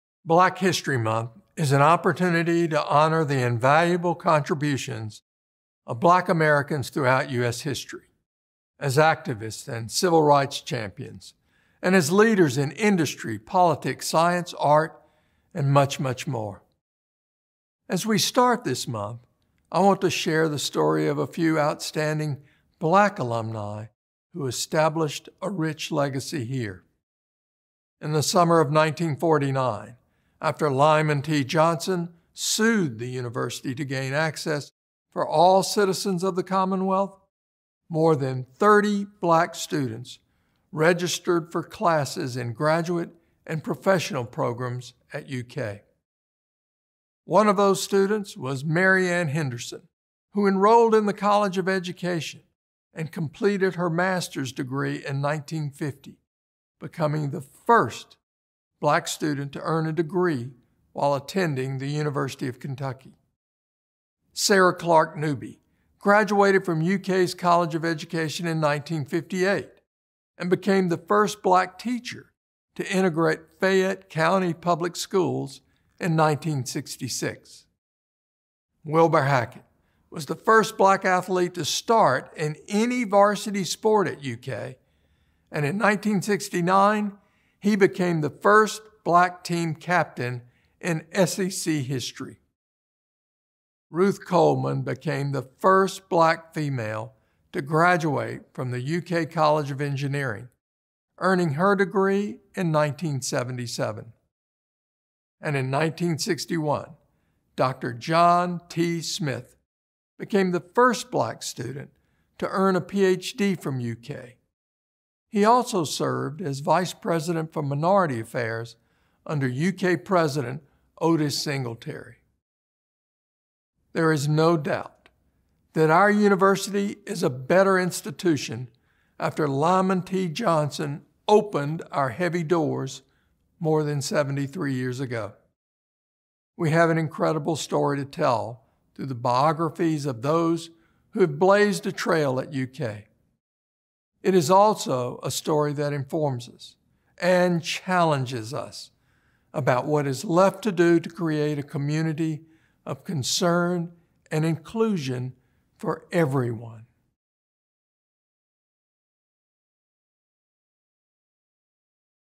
Celebrating Black History Month at the University of Kentucky
University of Kentucky President Eli Capilouto shares more about some key Black alumni who broke barriers on our campus as well as in our community and country.

Special thanks to UK Special Collections, UK College of Engineering, UK Athletics and UK Alumni Association Pioneer Project for photos used in this video.